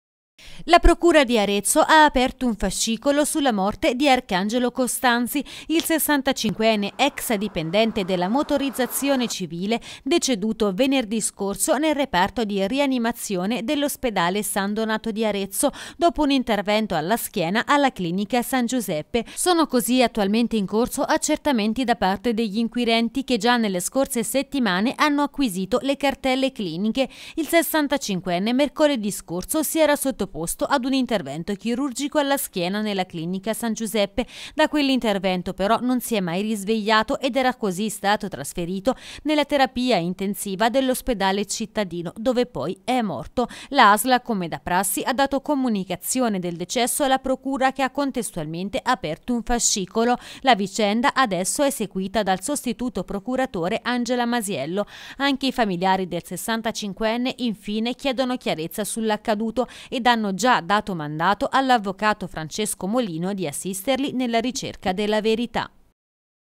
Arezzo, morto dopo un'operazione alla schiena, la Procura apre un'inchiesta
La procura di Arezzo ha aperto un fascicolo sulla morte di Arcangelo Costanzi, 65enne, l'ex dipendente della Motorizzazione Civile deceduto venerdì scorso nel reparto di rianimazione dell’ospedale San Donato dopo un intervento alla schiena alla clinica San Giuseppe.
Sono in corso accertamenti da parte degli inquirenti che nelle scorse ore hanno acquisito le cartelle cliniche.